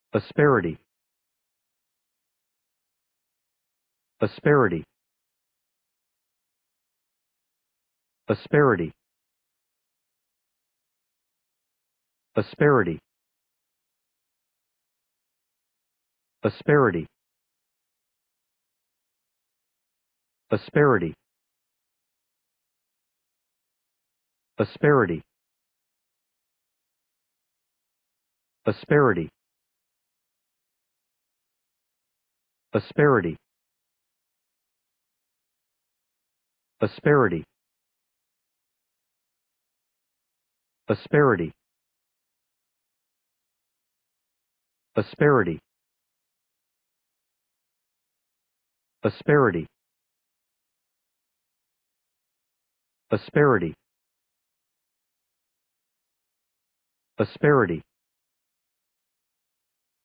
How to pronounce Asperity | English pronunciation
Learn how to pronounce Asperity in English
ASPERITY
Pronunciation of Asperity: /ə'sperɪtiː/
Definition of Asperity: (1) (noun) harshness of manner; (2) (noun) something hard to endure;